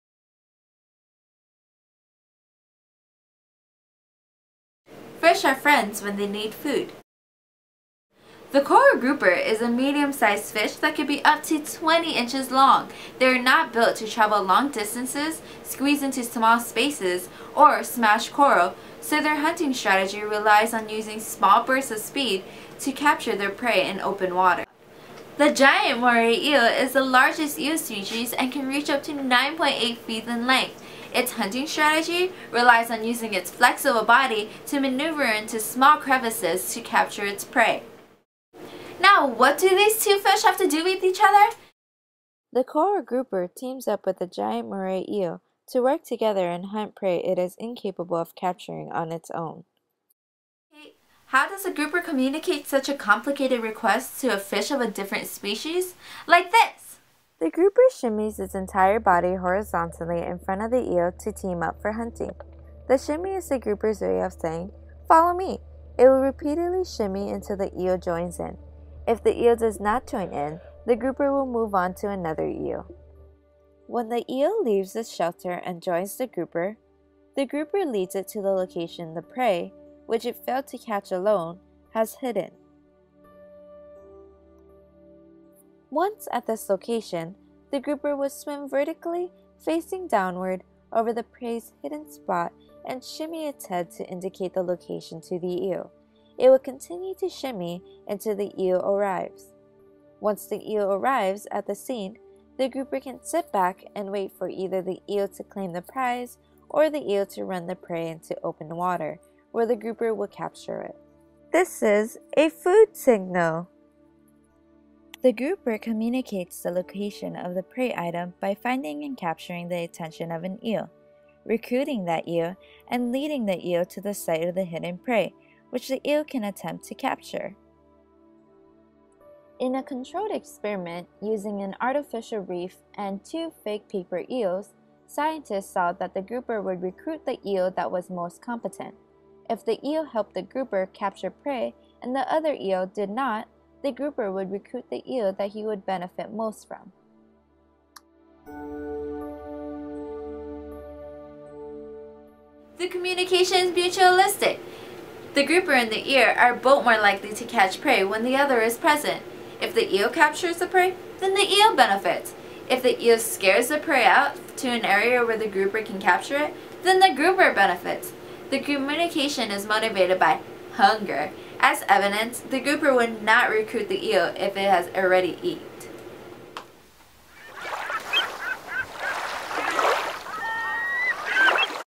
Interspecies Communication Between the Grouper and the Eel
All videos displaying the fish were taken from
Bshary R, Hohner A, Ait-el-Djoudi K, Fricke H (2006) Interspecific communicative and coordinated hunting between groupers and giant moray eels in the Red Sea. PLoS Biol 4:e431
and
Nature Video - Clever Fish: Cooperation on the reef